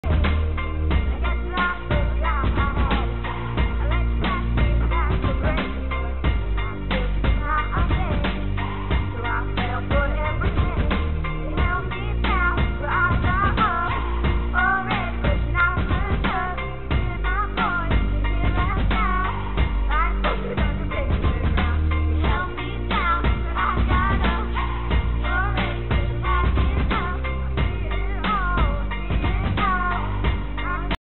Karaoke Shower, Childwall Academy, Liverpool
We want you to sing your heart out and impress your friends with your shower show stopping tunes! Shower superstars will be able to share and watch back their performances with friends and family via our YouTube channel whilst learning the importance of copyright to the music they love. The Karaoke Shower is a focal point of the Treasure Island campaign visiting UK towns and cities.